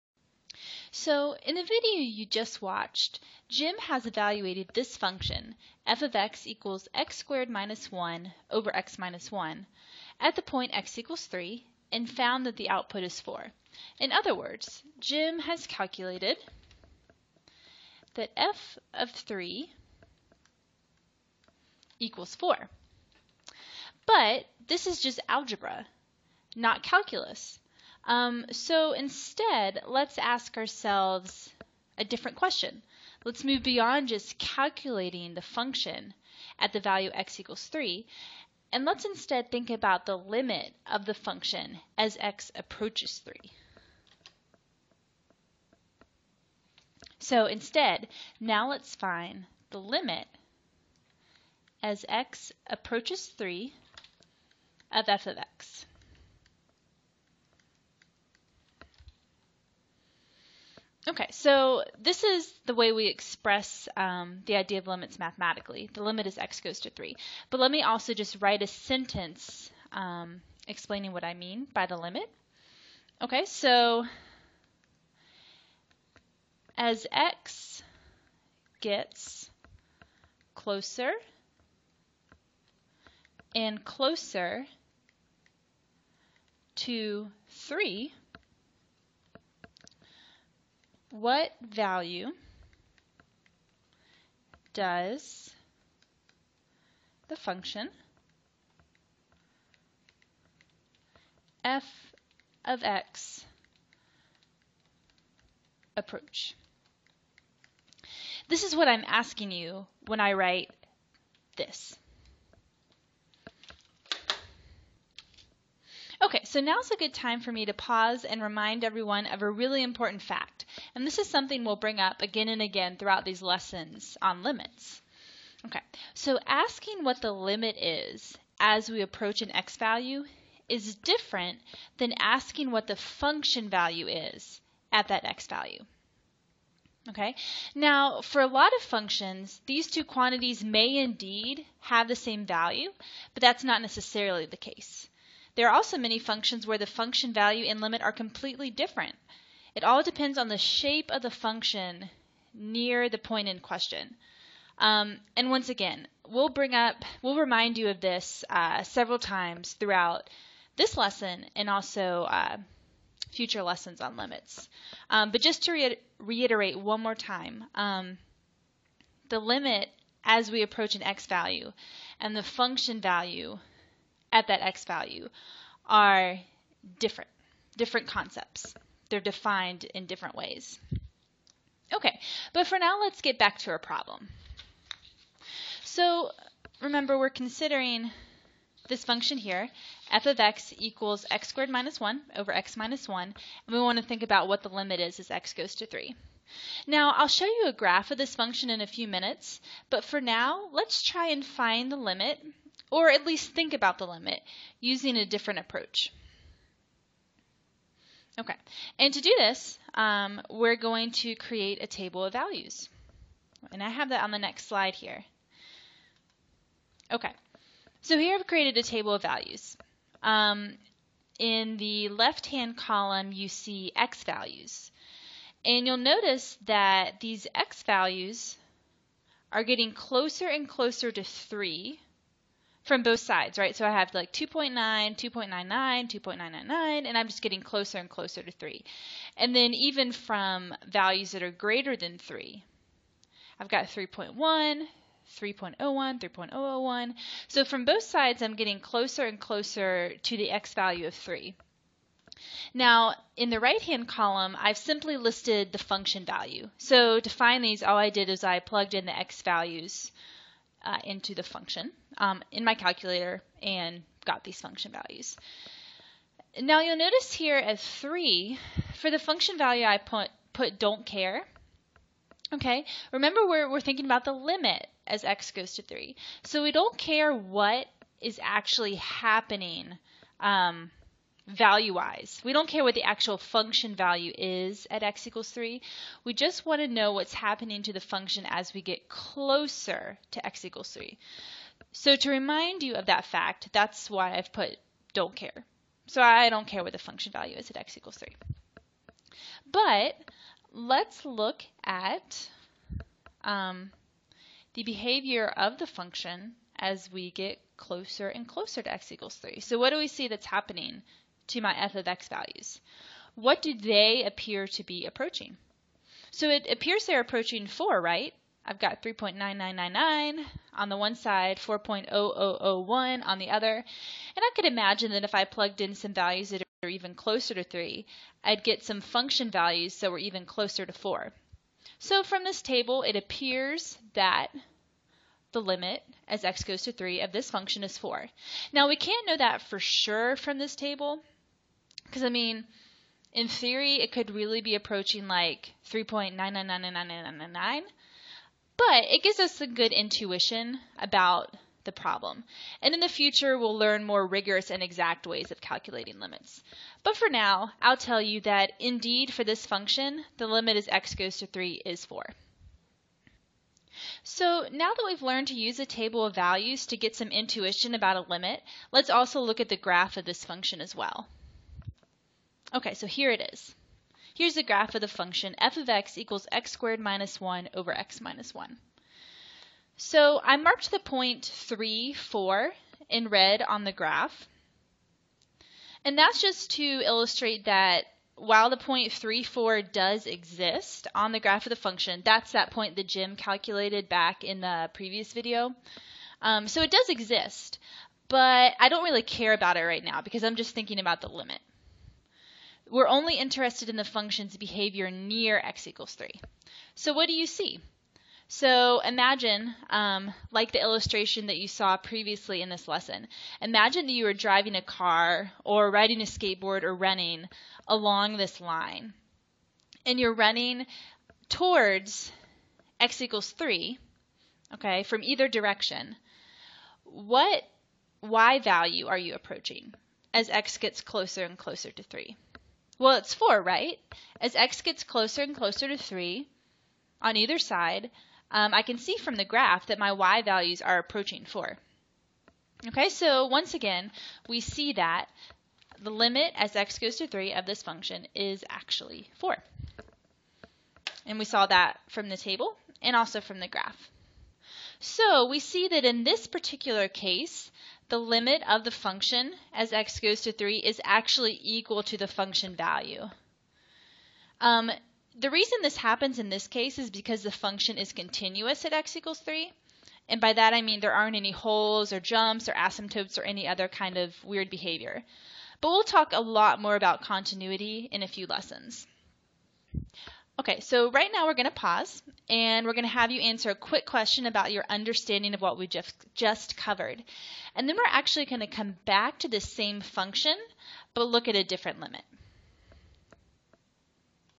Estimating_Limits
Learn how to estimate the limit of a function value by plugging in nearby numbers, and how the limiting value is different from the function value.